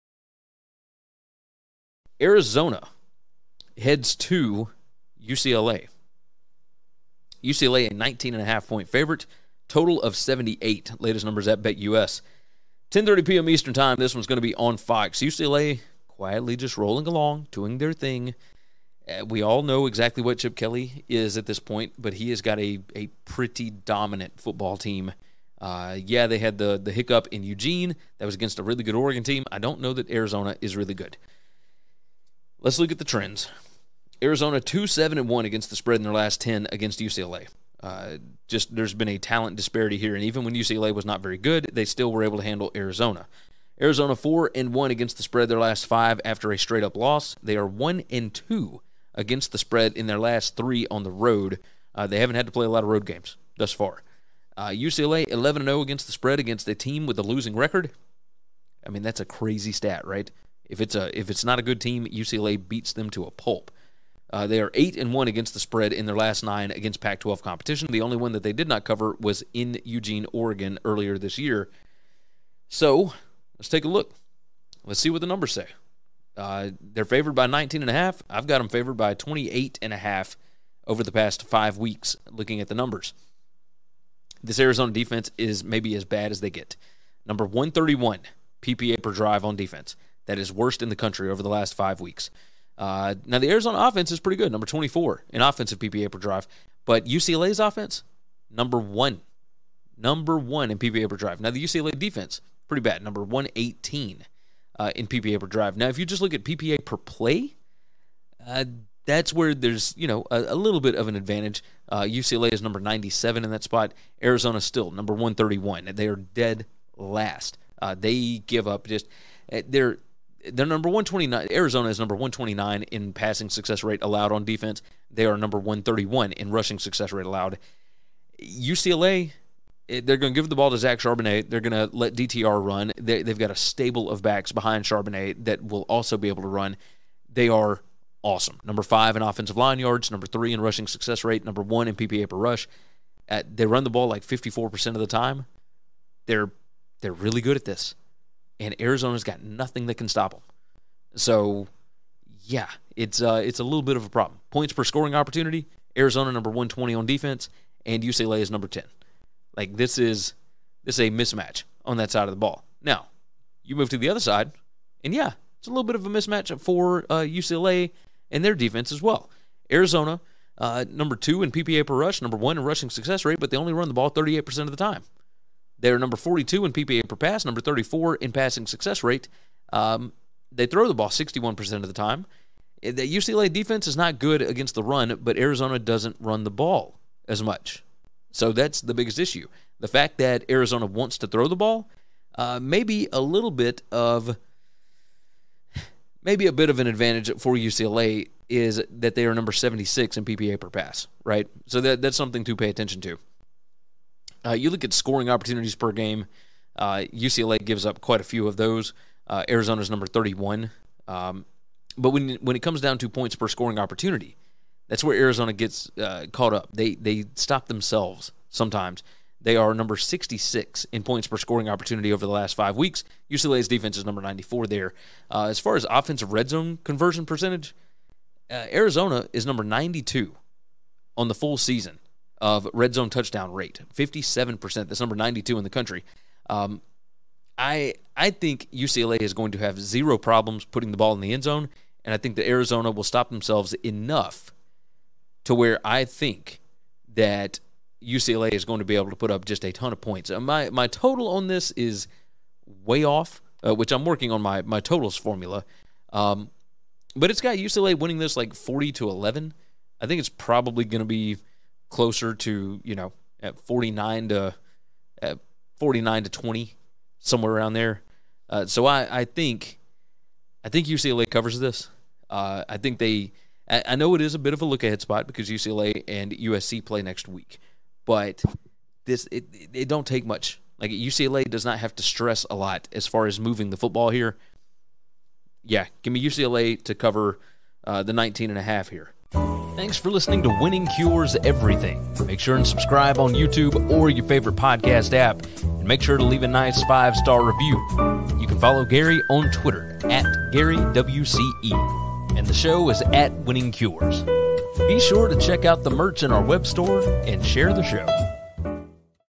UCLA vs Arizona Picks & Prediction Against the Spread 2022 College Football Analysis
UCLA vs Arizona Picks Against the Spread 2022 Predictions

Gary gives a quick preview, analysis, prediction & his pick (lean) against the spread (ATS) for the 2022 College Football Week 10 matchup featuring Arizona vs UCLA!